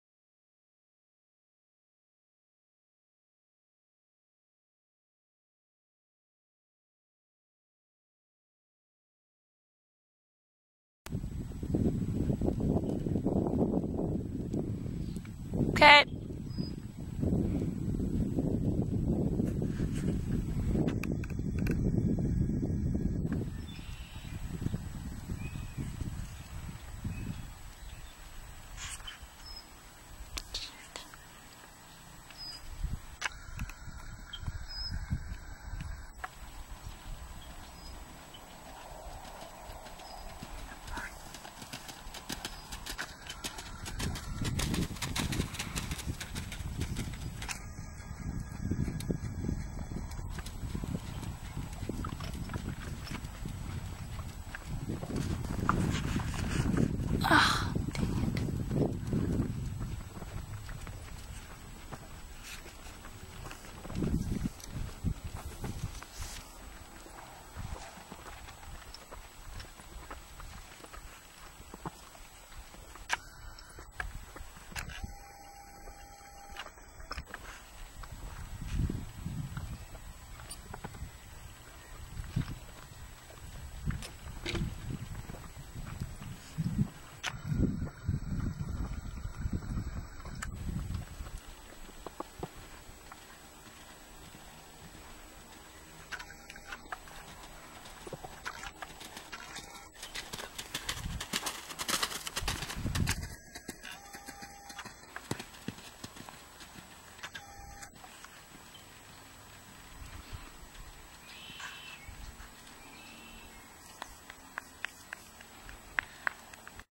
Sadie - Draft Cross for sale in Idaho
Sadie is one of those special draft cross horses. She is kind and gentle and always willing. She has SMOOTH gaits...especially the canter. On the day this video was taken, I had loaned my saddle to a friend so I was riding Sadie bareback. We were cantering around so comfortably that we decided to take her over a few small jumps. Sadie was a super star. She had no problem with the fences. In time, she will be a true packer. She is perfect for someone looking for a long-term horse. A horse that will be enjoyed by many levels of riders. Sadie is not a clunky draft cross. She is leggy and smooth built. She has a long, easy stride, and jumps with ease. She is naturally balanced will collect. She will master lead changes and some of the more advanced moves.Sadie has been sold and is in training for Eventing! We miss this sweet lady but she is doing well and is a barn favorite because of he sweet personality.